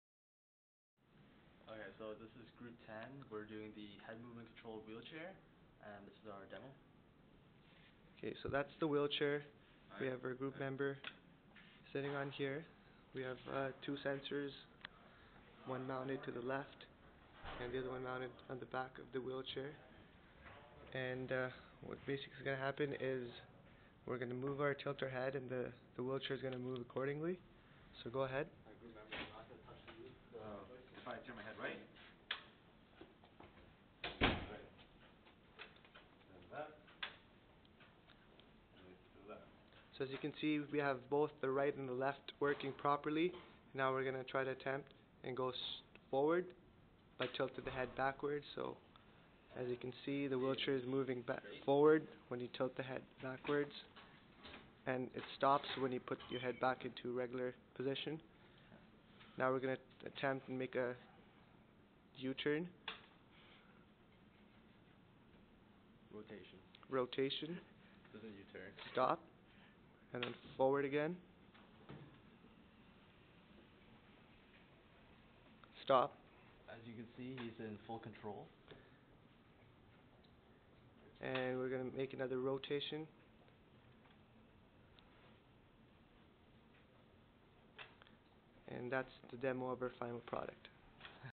Head Movement Controlled Wheel Chair - Final testing phase
This wheelchair is designed for quadriplegic patients that cannot move their limbs and therefore aren't able to control the joystick for normal electric wheel chair. This wheel chair works with no touch head movement using ultrasound sensors